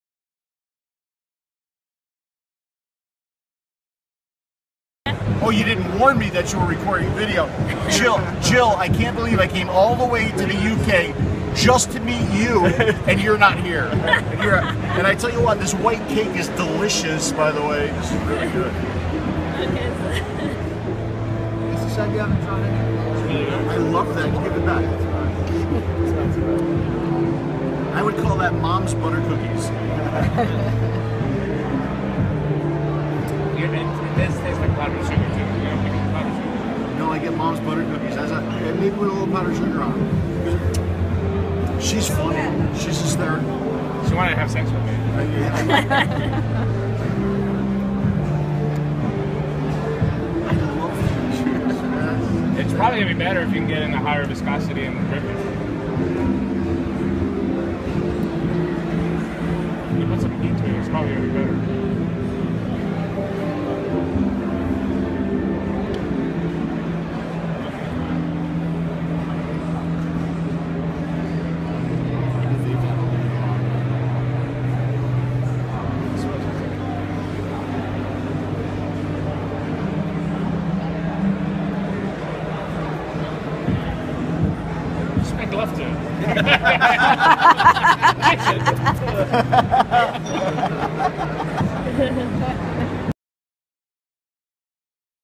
Ο Phill Busardo Για Το White Cake Υγρό Αναπλήρωσης
Η γνώμη του Phill Busardo για το υγρό αναπλήρωσης από την myVape, White Cake